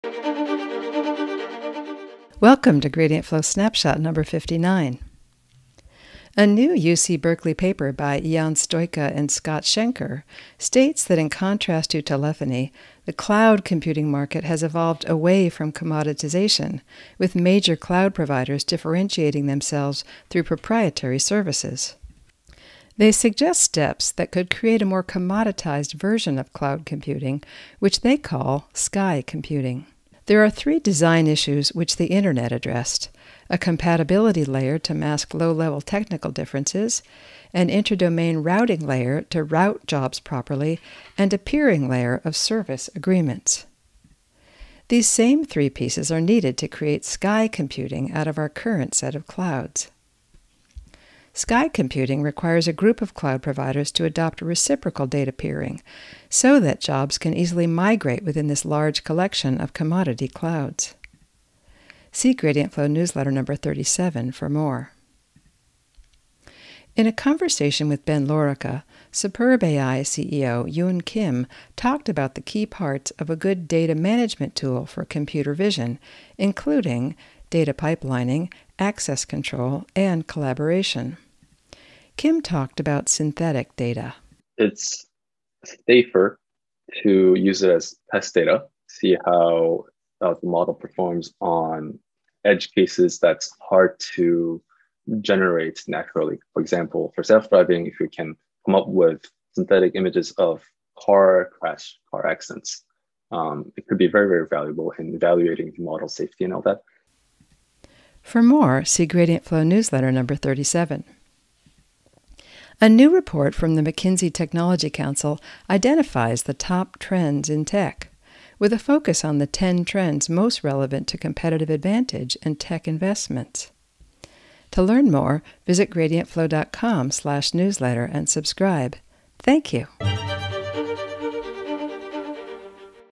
Gradient Flow Snapshot #59: From Cloud → Sky computing; Automation in DataOps; Top Technology Trends
- From Cloud Computing to Sky Computing
- Automation in Data Management and Data Labeling
- The Top Trends in Tech: A must-read new report and interactive website from the McKinsey Technology Council